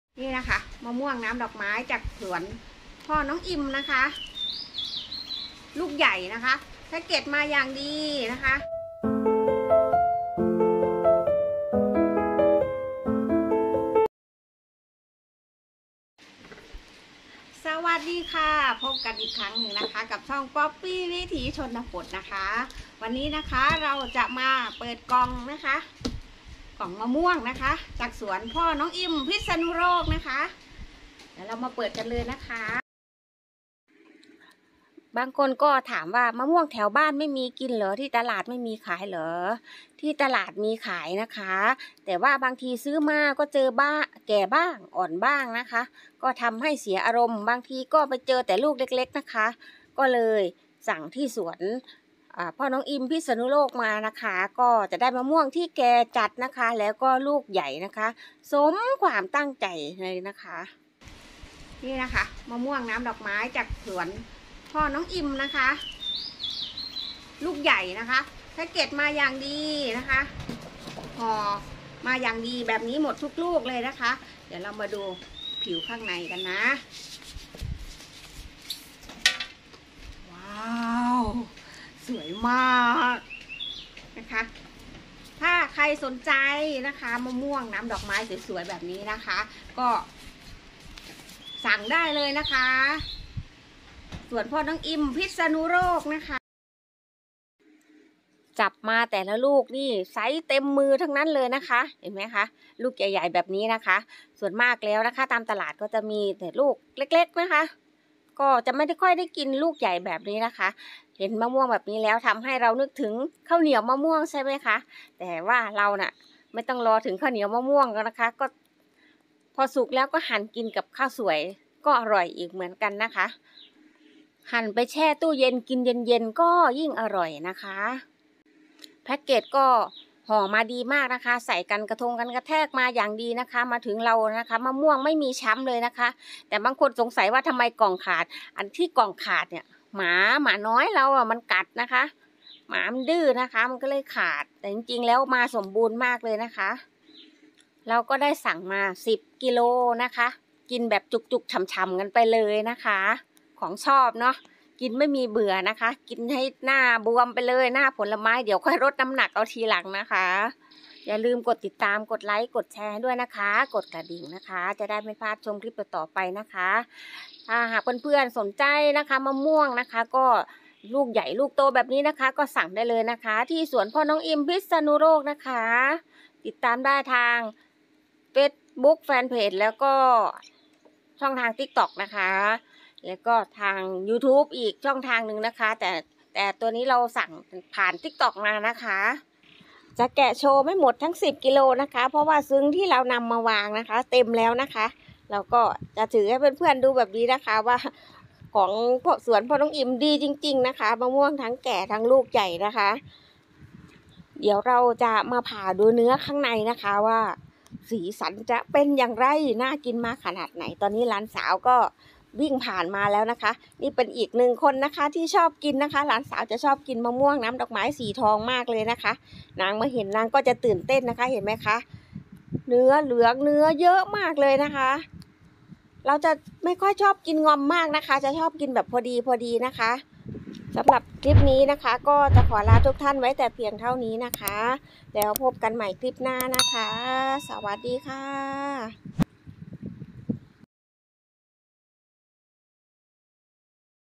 มะม่วงน้ำดอกไม้สีทอง สวนพ่อน้องอิมพิษณุโลก ลูกใหญ่แก่จัด
มะม่วงน้ำดอกไม้สีทอง สั่งมาจากสวนพ่อน้องอิมพิษณุโลก ลูกใหญ่แก่จัด
ห่อมาให้อย่างดีทุกลูกเลยค่ะ
ช่องทางติดต่อเรา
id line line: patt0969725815
ขอบคุณทุกท่านที่ติดตามรับชมค่ะ